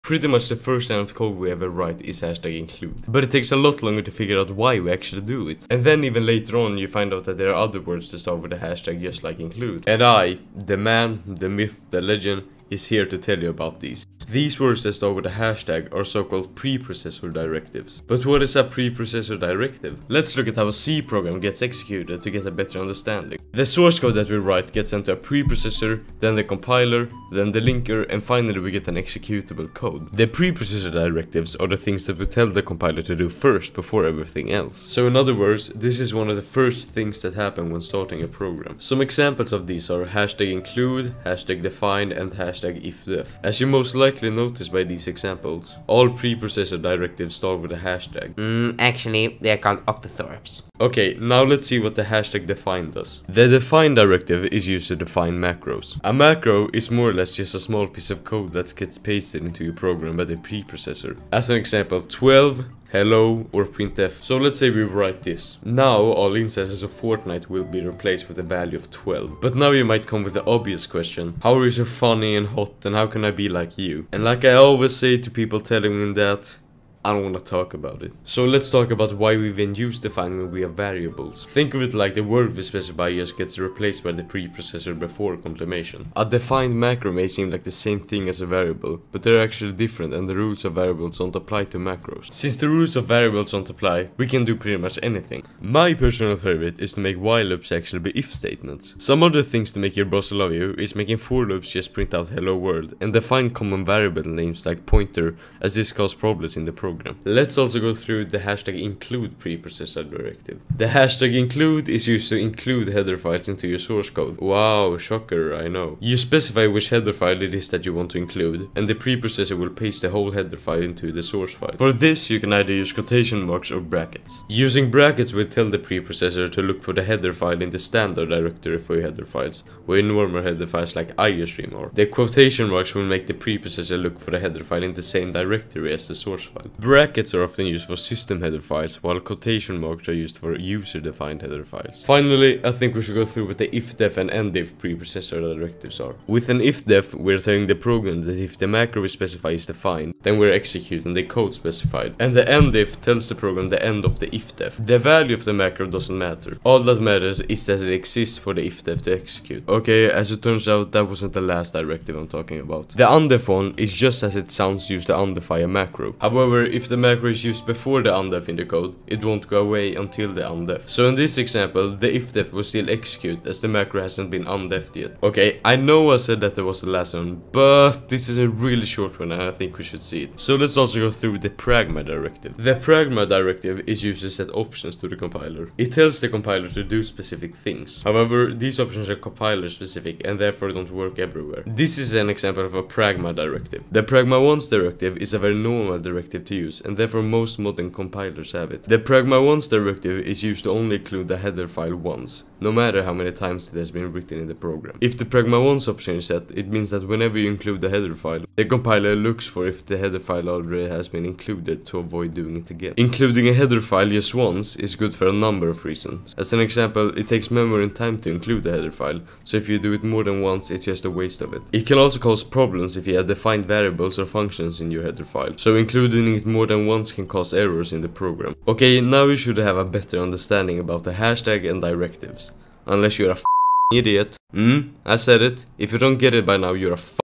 What are preprocessor directives in C and C++?
Welcome to the best video you have ever seen.
It is about what preprocessor directives like include , define and pragma are.